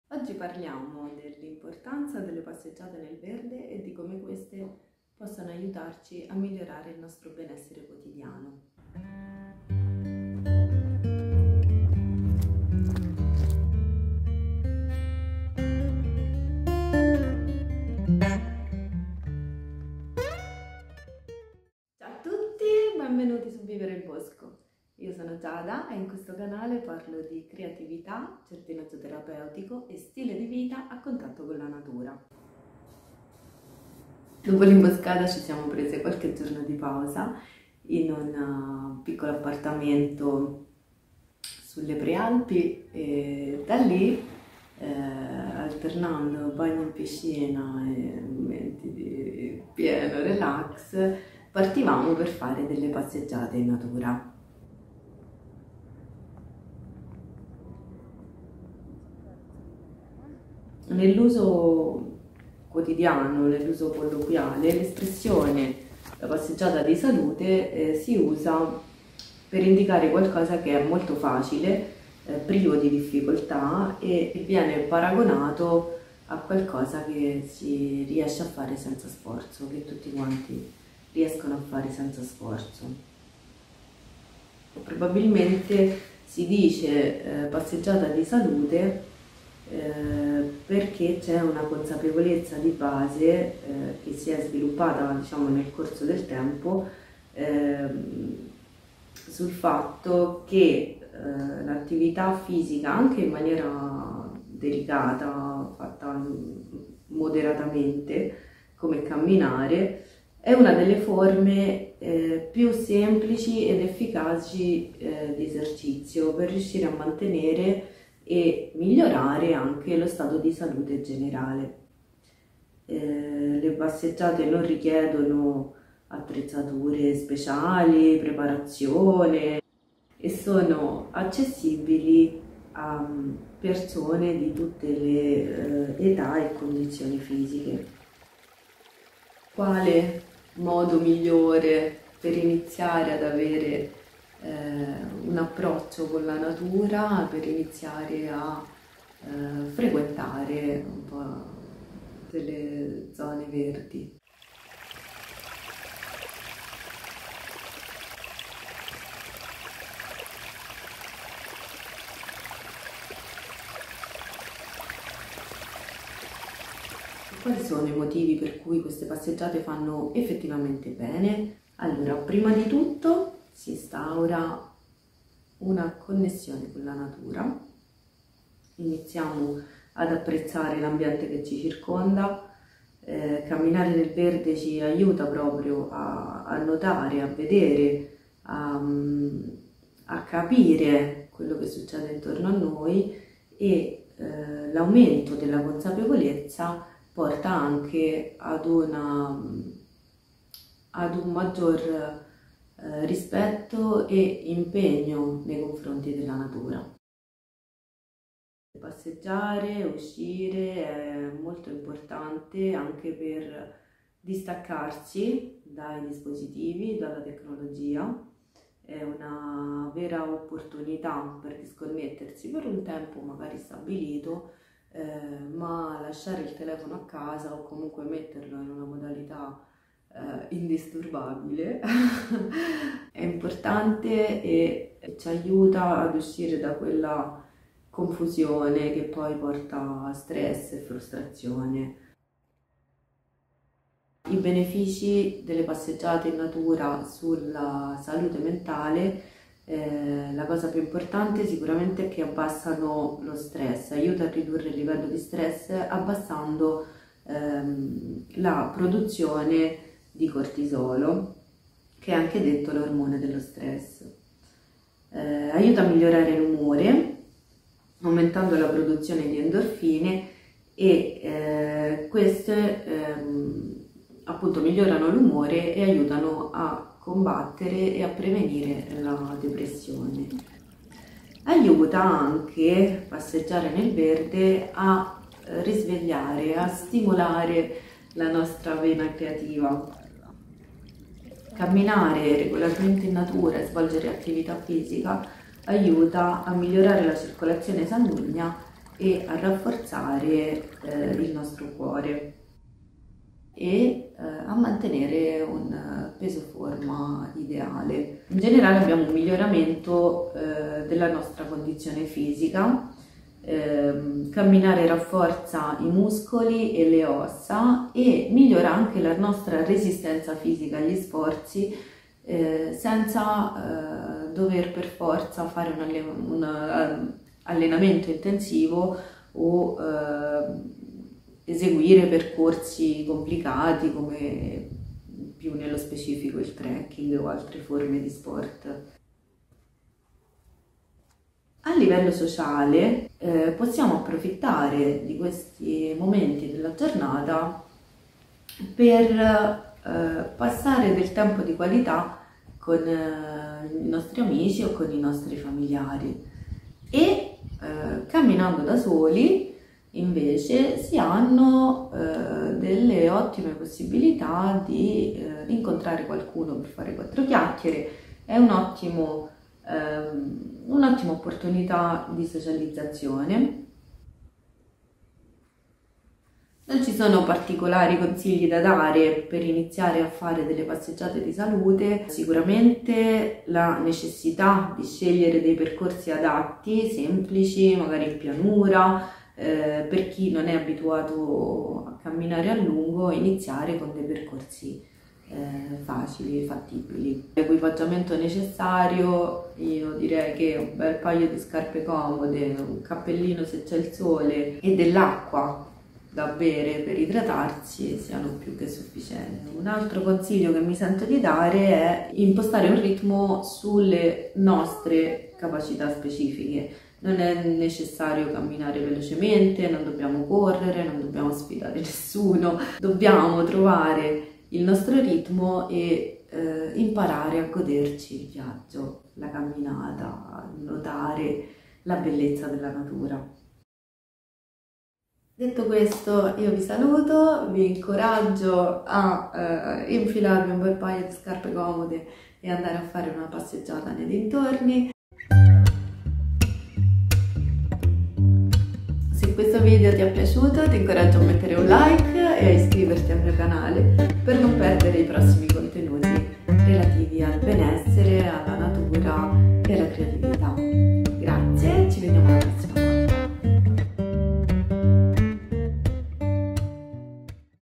La passeggiata di salute: perché ci fa bene camminare nel verde?🌲Benefici e consigli per iniziare.
🌸 Benvenuti su VIVERE IL BOSCO! In questo video, esploreremo insieme i molteplici benefici fisici, mentali ed emotivi delle passeggiate nel verde e scopriremo come iniziare a fare delle passeggiate di salute una parte della nostra routine quotidiana.

🌸 Cerchiamo di capire insieme come una semplice passeggiata nel bosco, in un parco cittadino o in qualunque contesto in cui ci sia del verde, può trasformare la tua vita e migliorare il tuo benessere generale.